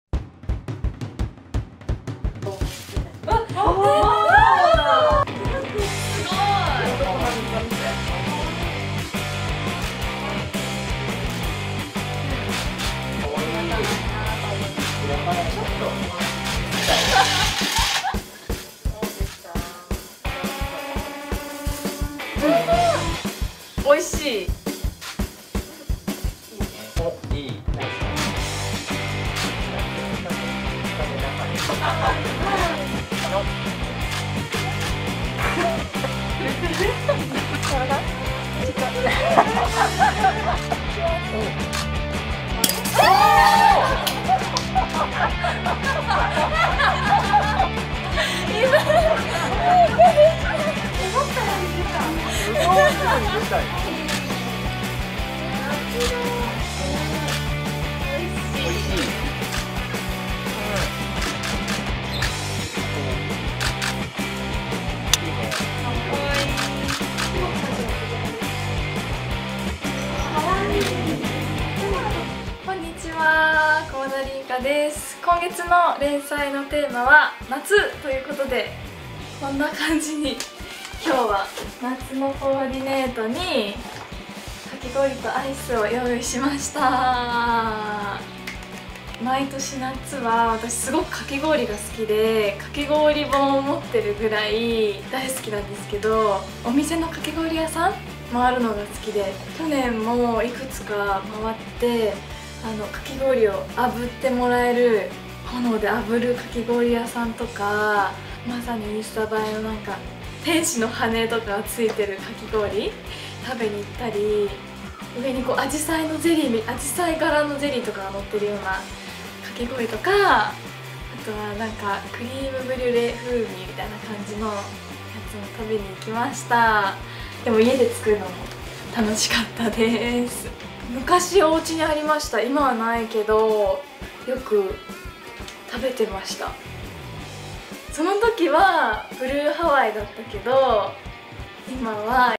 りんくまの夏は、かき氷。好きな味と思い出を語る
セブンティーン連載「りんくまがじん」第27回のテーマは、夏。アイスキャンディーやラムネ、かき氷を楽しみました☆　STアプリで視聴できる続きには、この日のメイク解説もあり！

▼無料アプリ「ST channel」のインストールはこちら